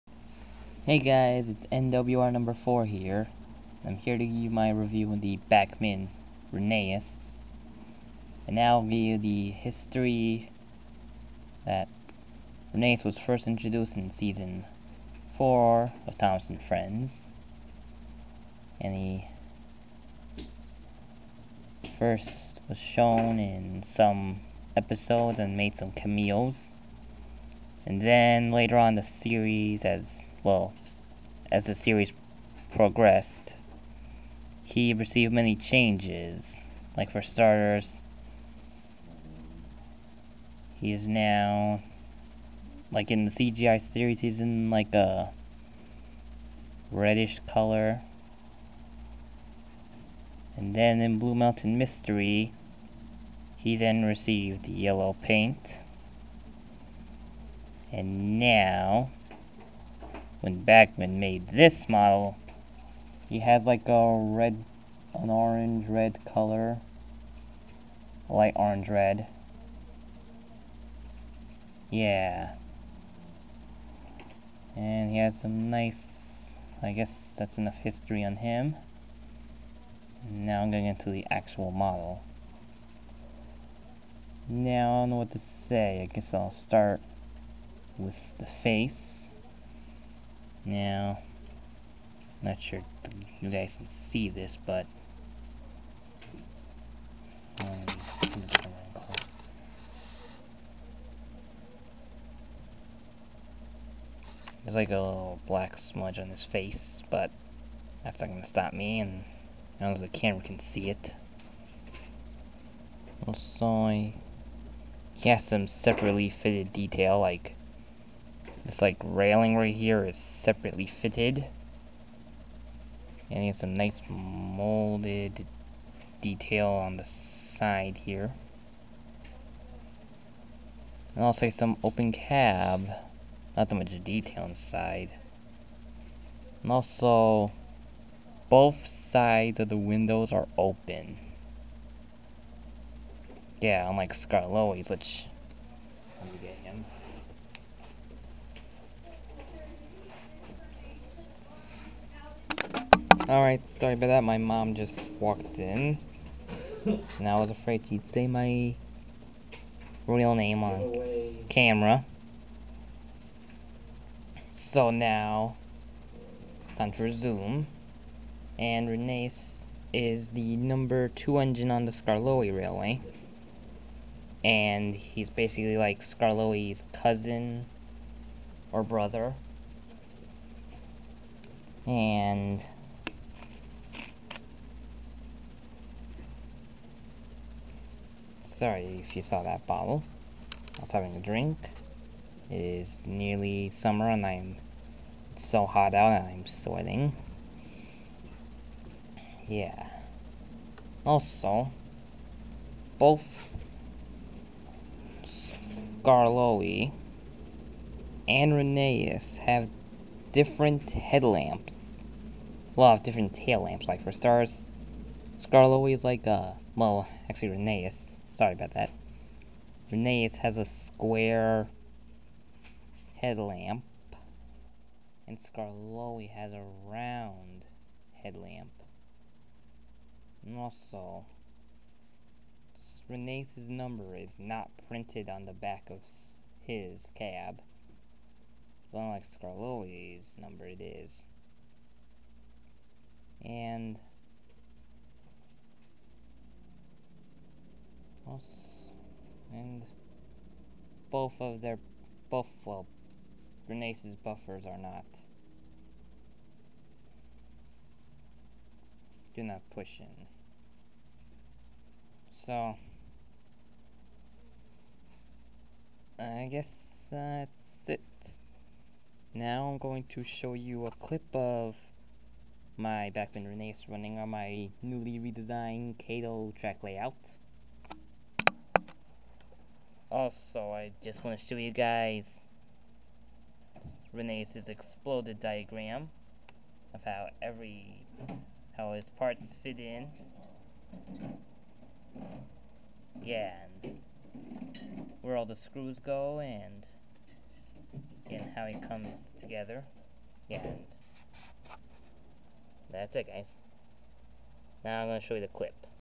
Bachmann Rheneas review
My second bachmann Thomas narrow gauge engine i was so happy to get this engine.